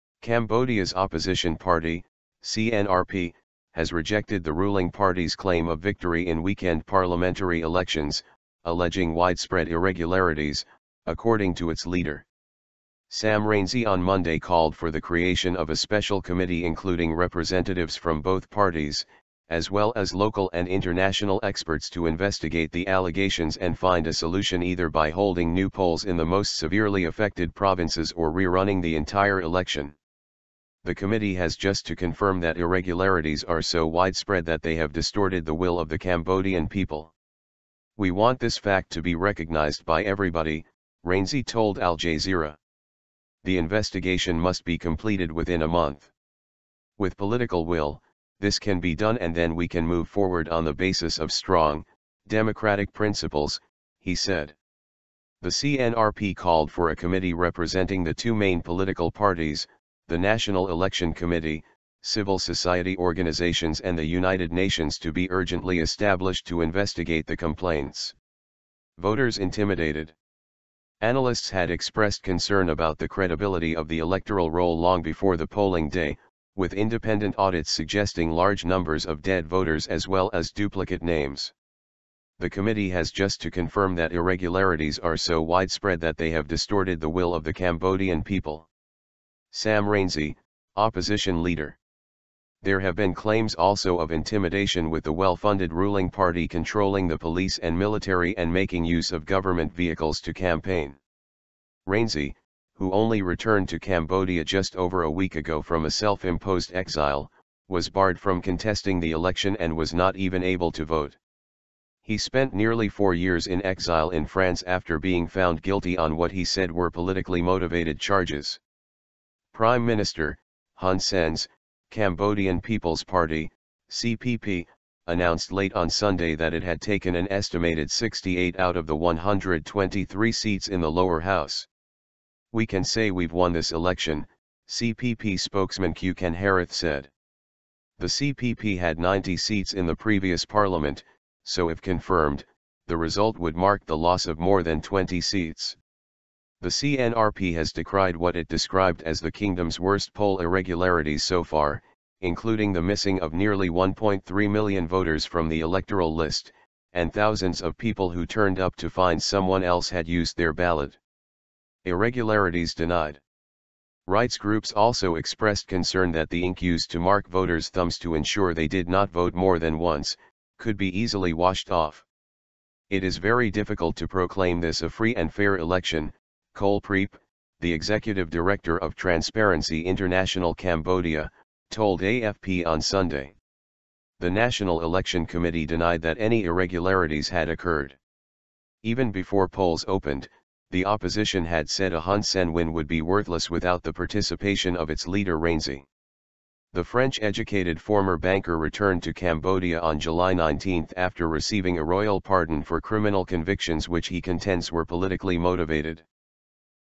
Cambodian Opposition Party Claims Corruption In Elections
Cambodia's opposition party (CNRP) has rejected the ruling party's claim of victory in weekend parliamentary elections, alleging widespread irregularities, according to its leader.

Sam Rainsy on Monday called for the creation of a special committee including representatives from both parties, as well as local and international experts to investigate the allegations and find a solution -- either by holding new polls in the most severely affected provinces or re-running the entire election.

"The committee has just to confirm that irregularities are so widespread that they have distorted the will of the Cambodian people. We want this fact to be recognised by everybody," Rainsy told Al Jazeera.

"The investigation must be completed within a month. With political will, this can be done and then we can move forward on the basis of strong, democratic principles," he said.

The CNRP called for a committee representing the two main political parties, the National Election committee, civil society organisations and the United nations to "be urgently established" to investigate the complaints.

Voters 'intimidated'

Analysts had expressed concern about the credibility of the electoral roll long before the polling day, with independent audits suggesting large numbers of dead voters as well as duplicate names.

The committee has just to confirm that irregularities are so widespread that they have distorted the will of the Cambodian people

Sam Rainsy, Opposition leader

There have been claims also of intimidation with the well-funded ruling party controlling the police and military and making use of government vehicles to campaign.

Rainsy, who only returned to Cambodia just over a week ago from a self-imposed exile, was barred from contesting the election and was not even able to vote.

He spent nearly four years in exile in France after being found guilty on what he said were politically-motivated charges.

Prime Minister, Hun Sen's, Cambodian People's Party (CPP) announced late on Sunday that it had taken an estimated 68 out of the 123 seats in the lower house.

"We can say we've won this election," CPP spokesman Khieu Kanharith said.

The CPP had 90 seats in the previous parliament, so if confirmed, the result would mark the loss of more than 20 seats.

The CNRP has decried what it described as the kingdom's worst poll irregularities so far, including the missing of nearly 1.3 million voters from the electoral list, and thousands of people who turned up to find someone else had used their ballot.

Irregularities denied

Rights groups also expressed concern that the ink used to mark voters' thumbs to ensure they did not vote more than once, could be easily washed off.

"It is very difficult to proclaim this a free and fair election," Kol Preap, the executive director of Transparency International Cambodia, told AFP on Sunday.

The National Election Committee denied that any irregularities had occurred.

Even before polls opened, the opposition had said a Hun Sen win would be "worthless" without the participation of its leader Rainsy.

The French-educated former banker returned to Cambodia on July 19 after receiving a royal pardon for criminal convictions which he contends were politically motivated.